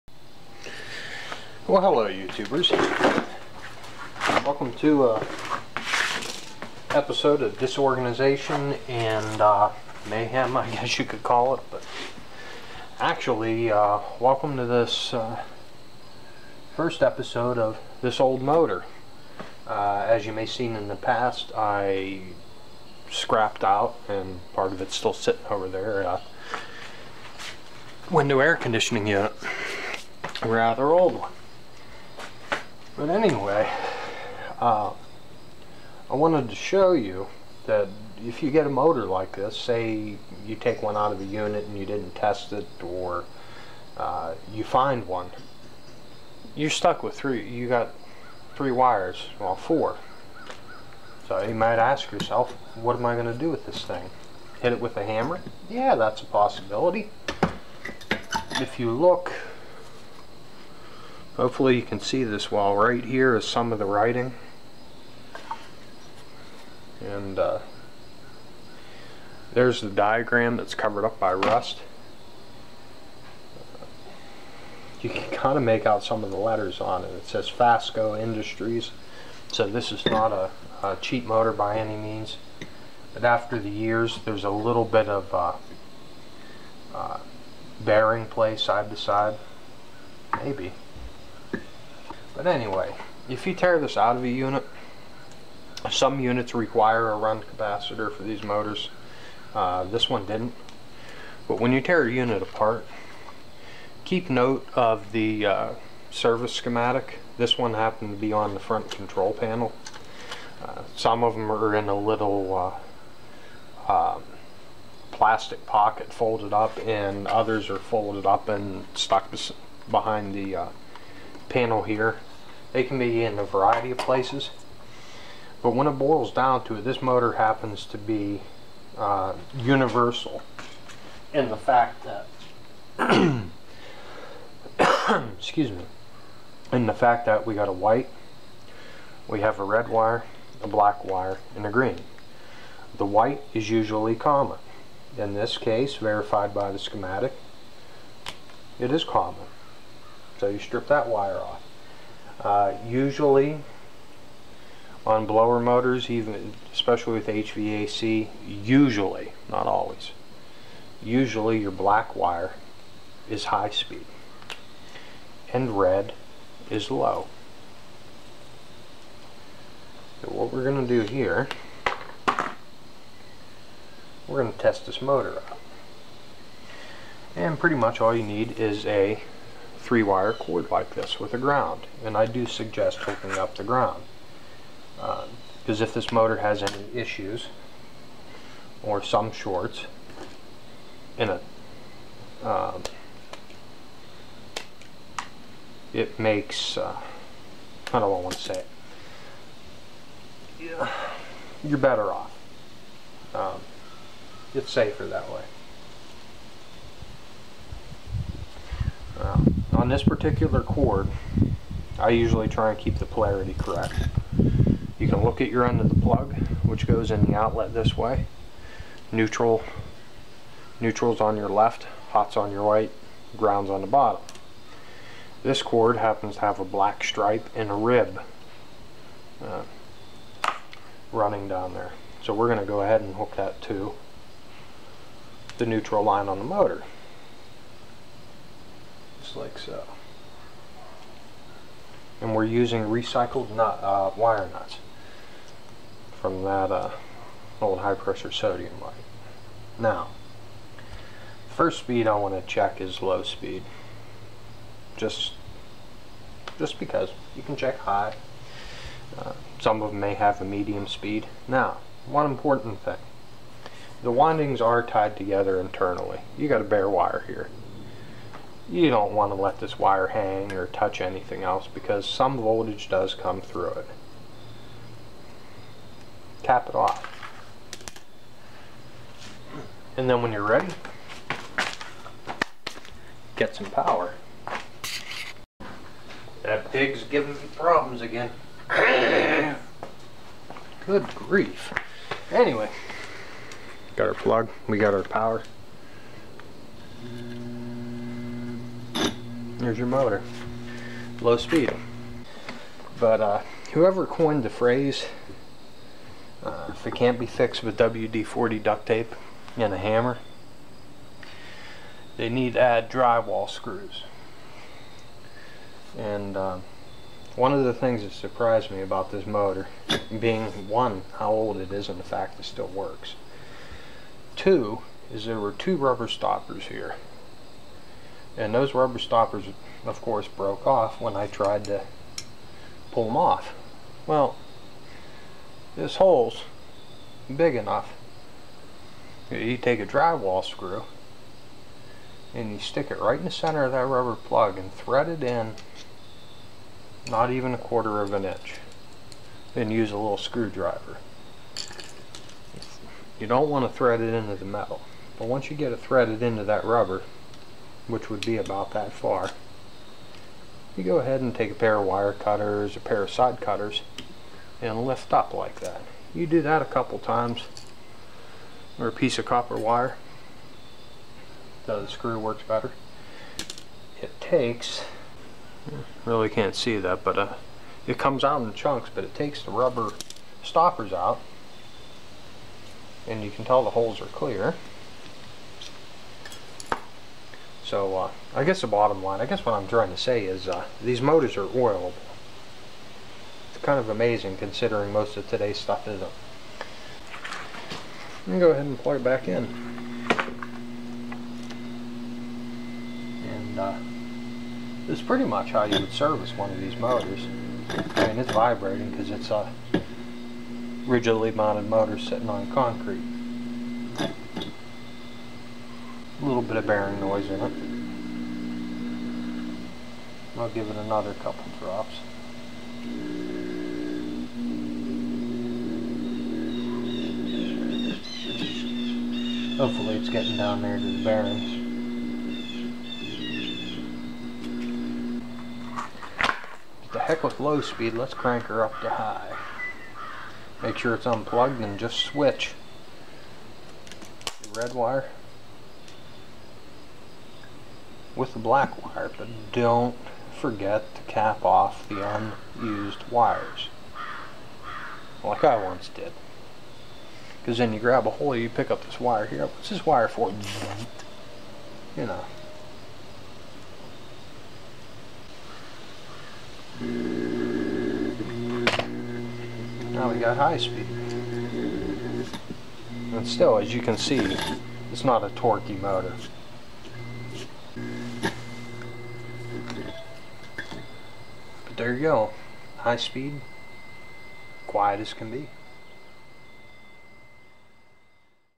How To Test - Air Conditioner Fan Motor
typical window unit fan motor